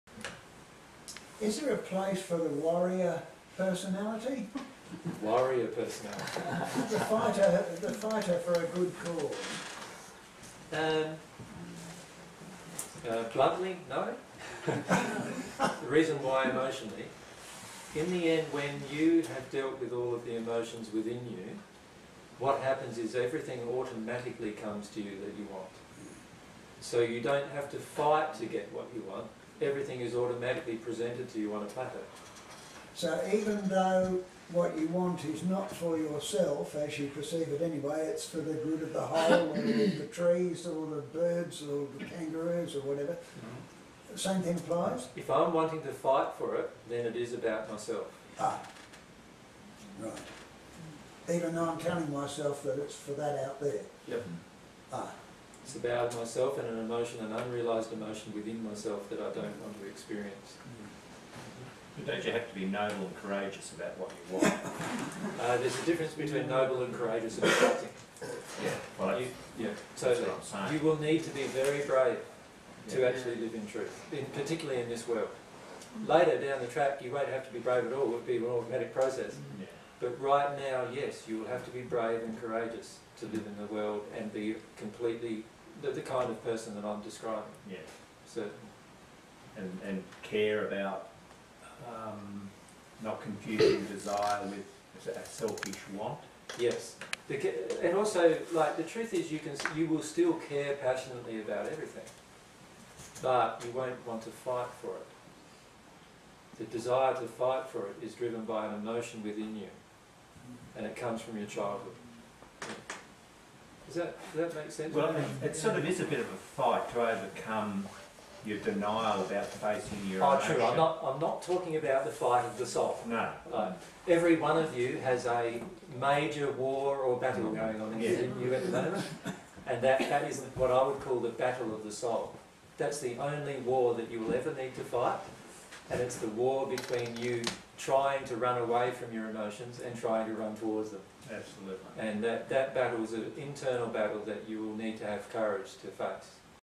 Jesus on The Only Fight You Will Ever Need to Fight
Jesus discusses fighting for causes.

This is a section of the presentation by Jesus entitled "20080928 General Discussion - Q&A From People In Lorinna S2".

dtstart:011310:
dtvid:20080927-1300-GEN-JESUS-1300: